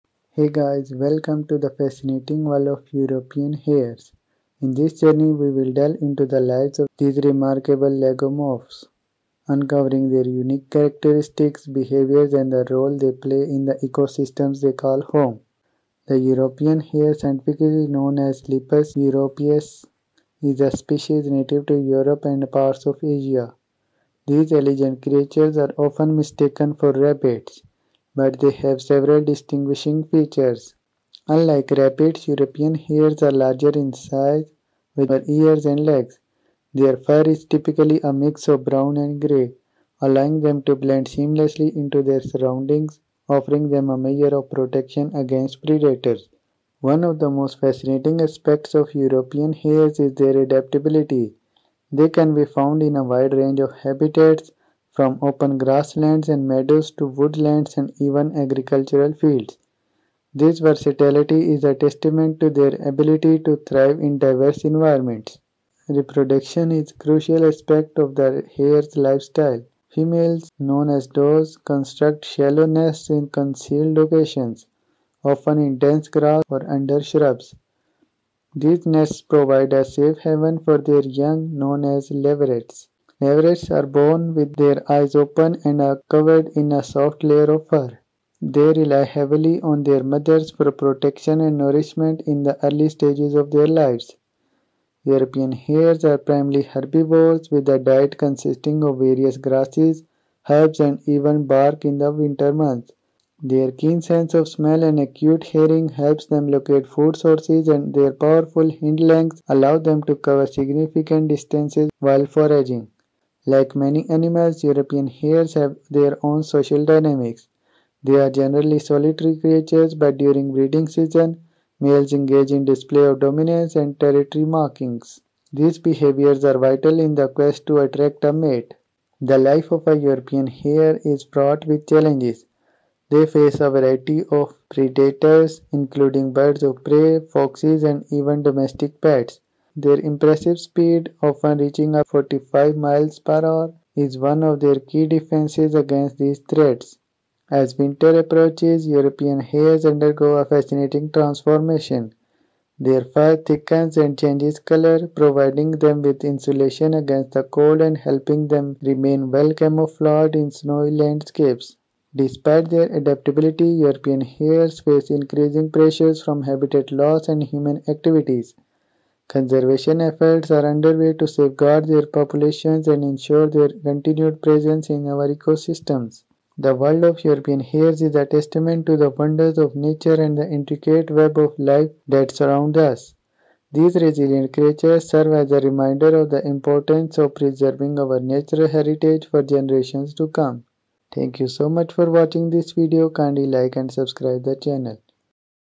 The Fascinating World of European Hares | An Animal Mistakenly Known As Rabbit | Paws and Claws
"The video, 'The Fascinating World of European Hares', offers an in-depth exploration of Lepus europaeus, commonly known as the European Hare. Discover their distinctive traits, adaptable habitats, and intriguing behaviors. From nesting habits to territorial displays, witness the challenges and triumphs of these remarkable lagomorphs. Gain insights into their critical role in the ecosystem and learn about ongoing conservation efforts. Join us on a captivating journey through the diverse and enchanting world of European Hares."

This channel is for interesting videos about the domestic and non domestic animals.